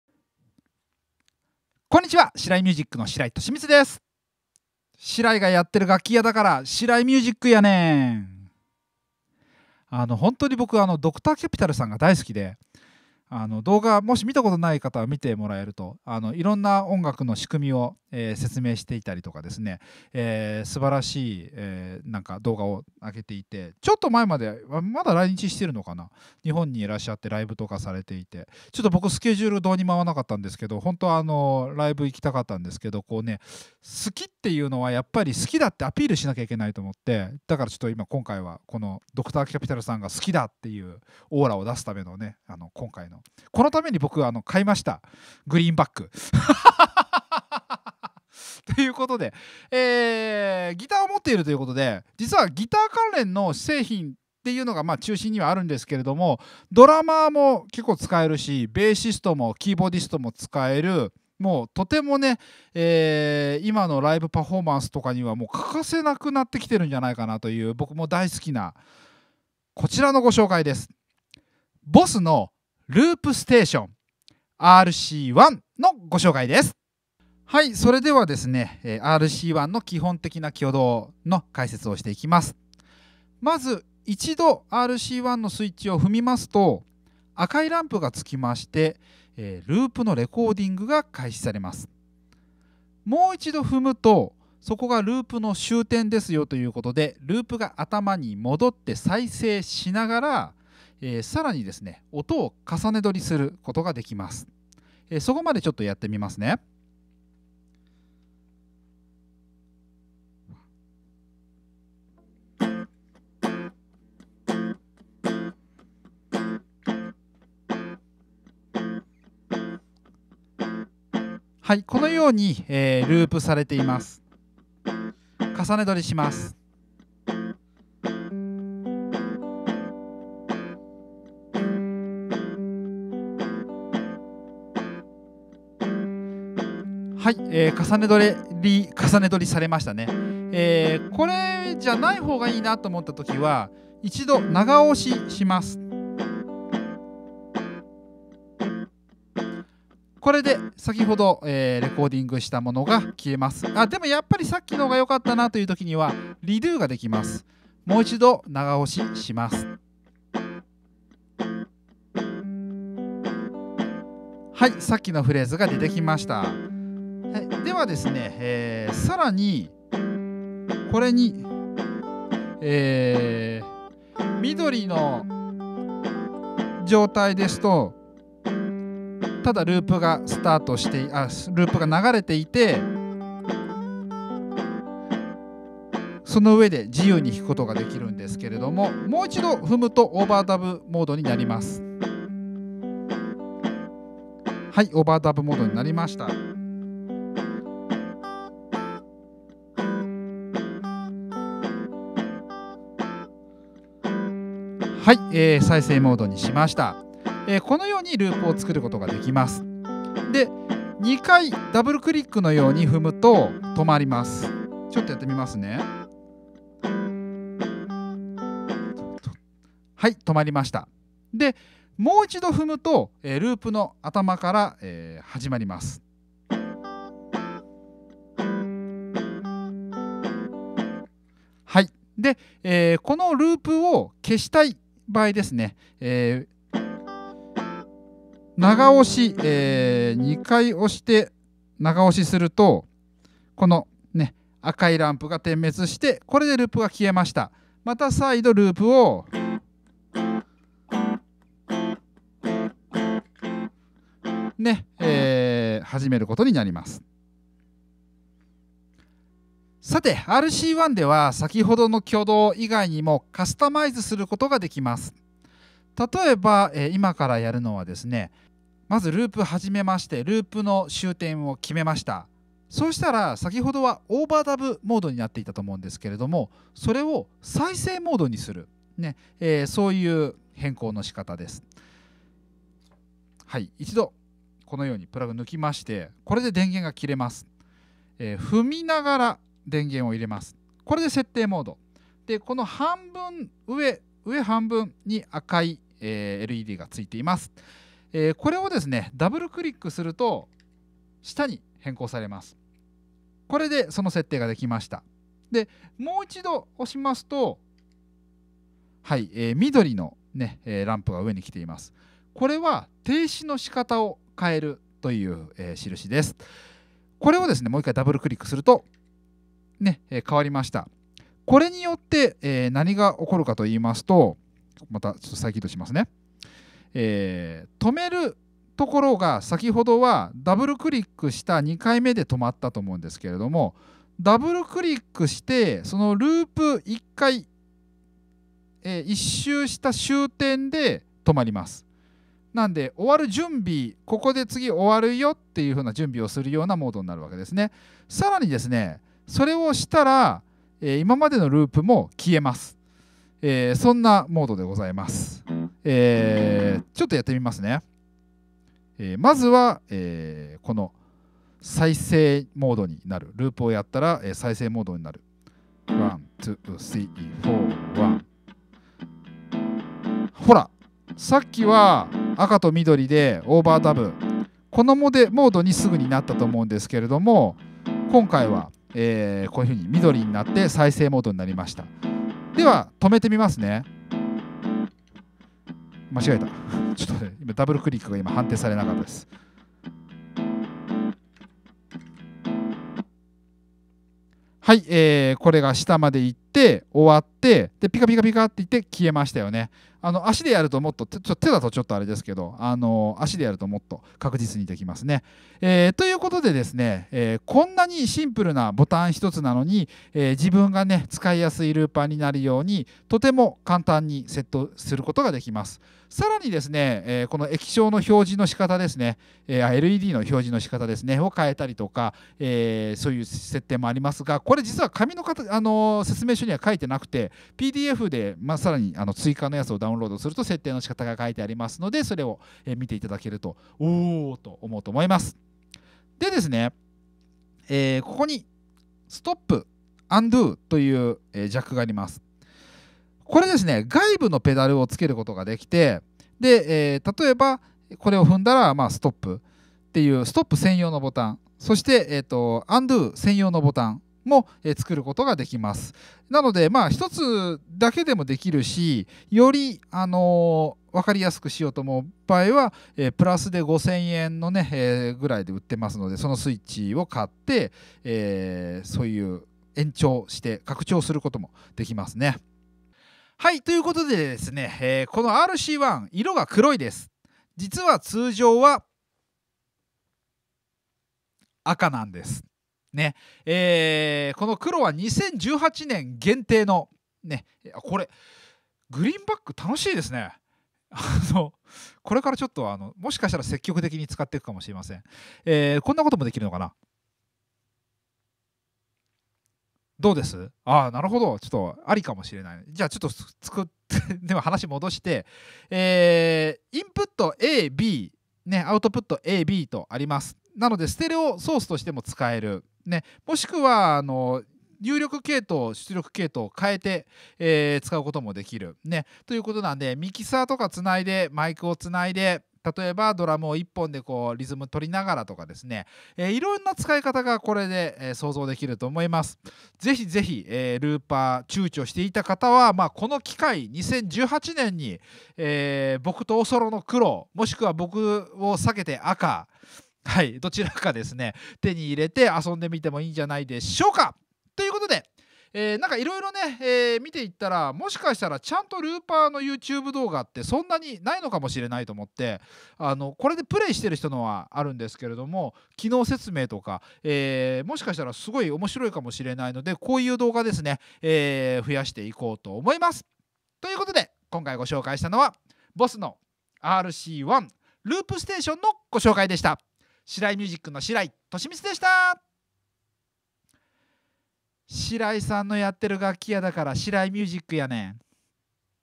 初めてのルーパーとして奥深く使える名機！BOSS Loop Station RC-1 BKの使い方解説動画です！
BOSS  Loop Station RC-1

あると便利な
BOSS (ボス) デュアル・フット・スイッチ FS-6
RC-10とスイッチの接続に
CANARE ( カナレ ) SPC01 / BK